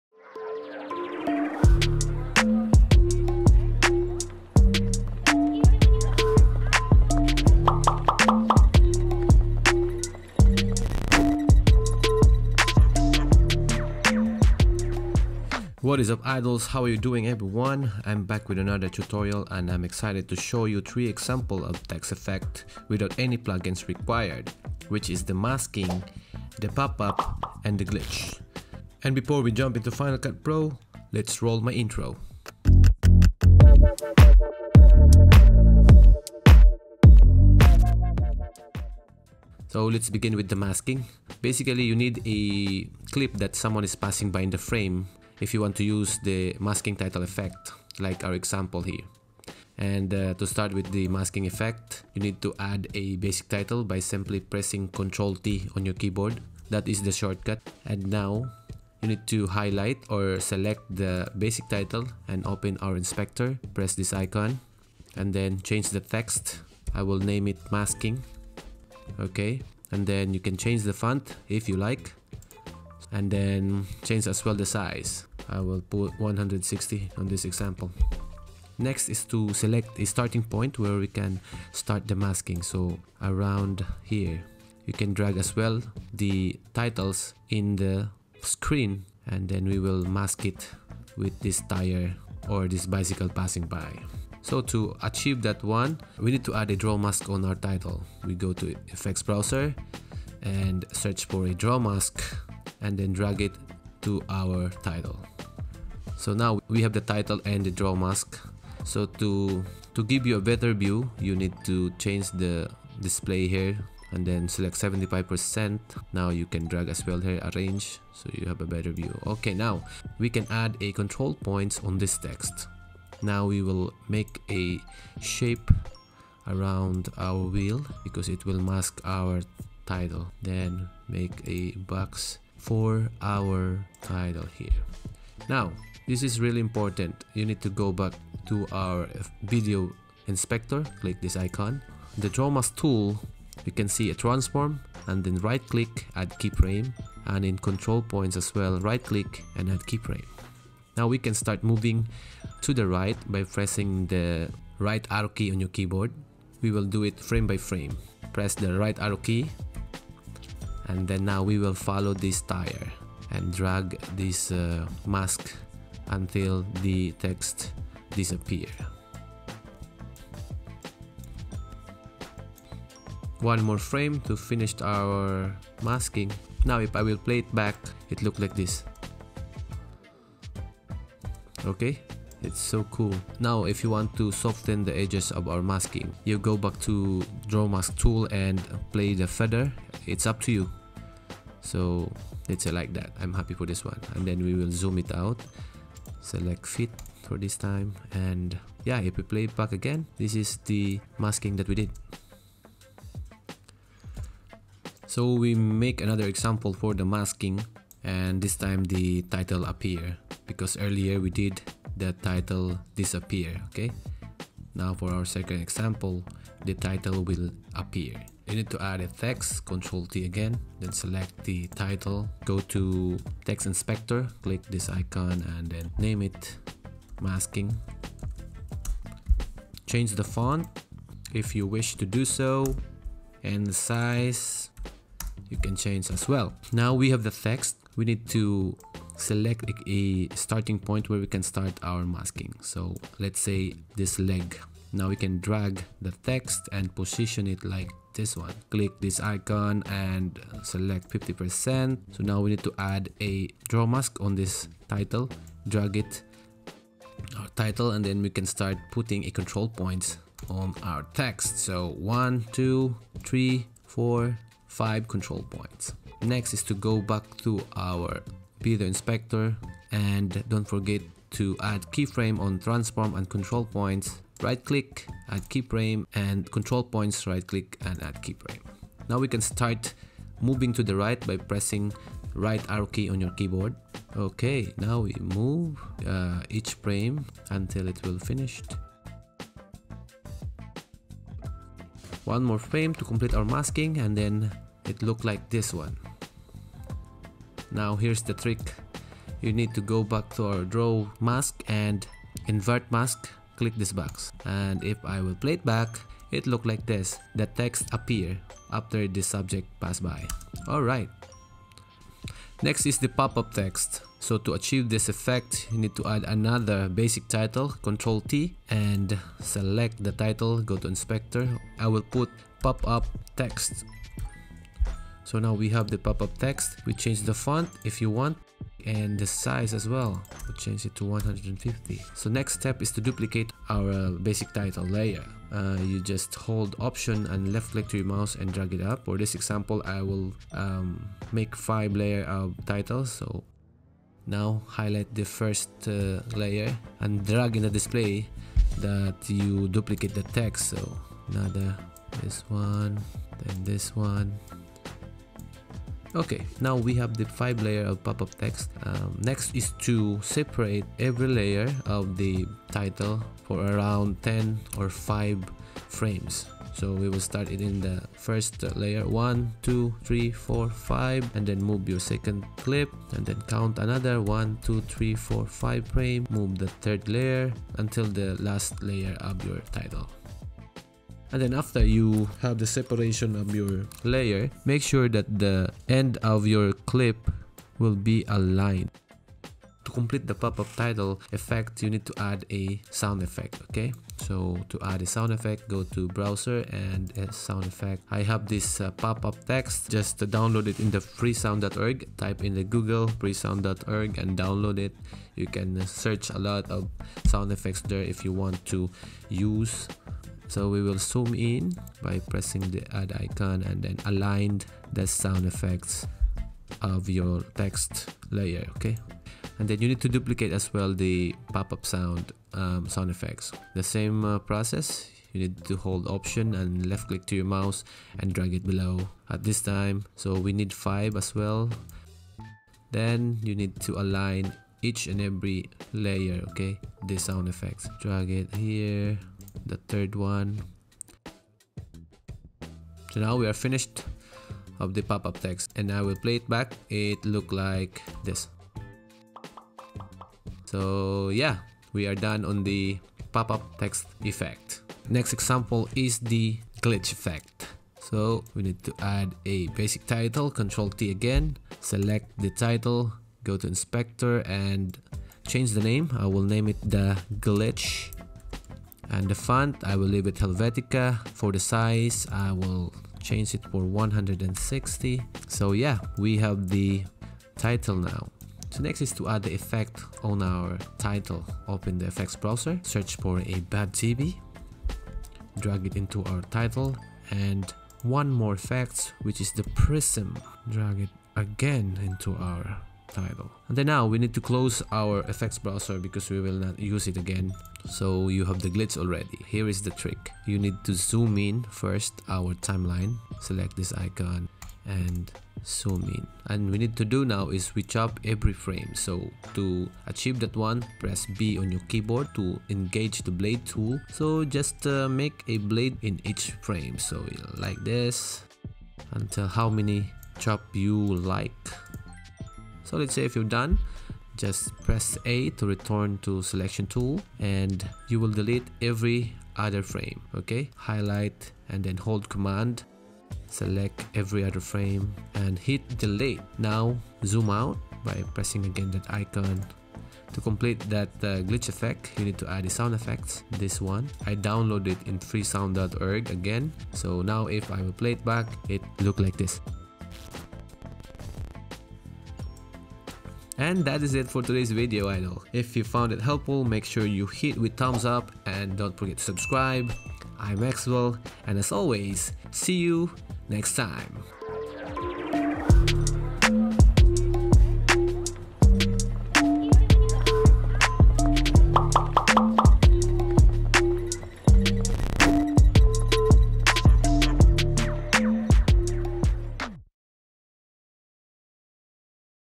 THREE USEFUL TEXT EFFECT IN FCPX- Tutorial (no plug-in)
Today I made a tutorial on how to make different types of text effects in FCPX and give you some IDEAS on how to mask, pop up and to glitch.